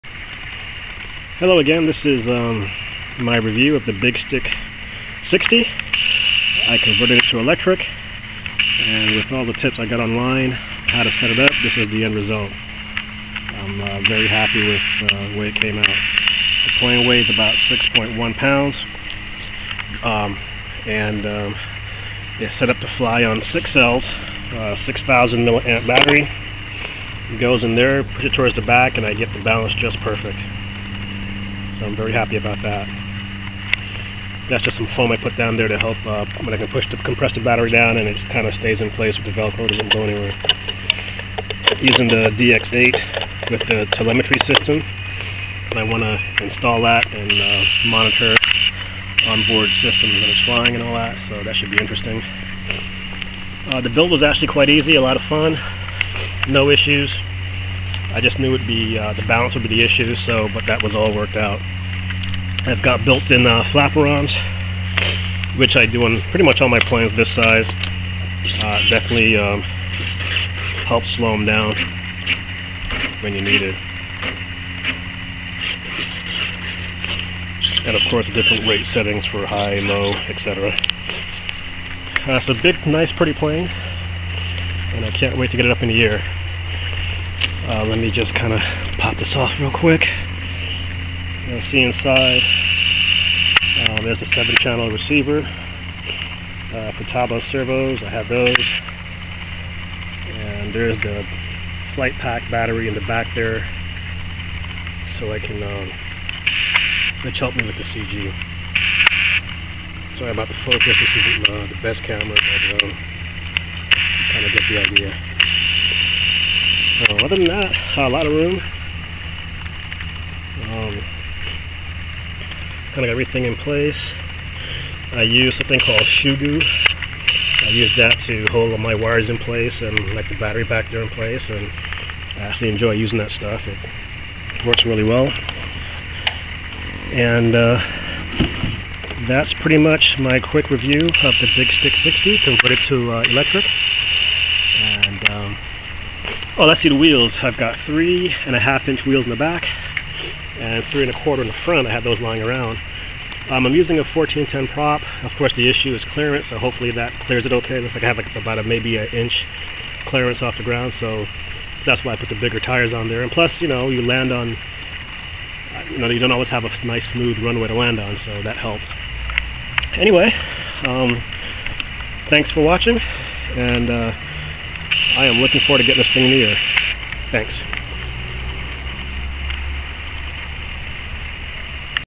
Big Stik 60 electric conversion.AVI
Electric conversion of the Big Stik 60. at 6.1 lbs. Rimfire 80 motor. 14x7 prop. Hobbywing 60 amp esc.  using 22v 5000 and 6000 mah batt. 7 ch receiver w/ telemetry. 2000 mah 6v batt on board. Maiden due soon.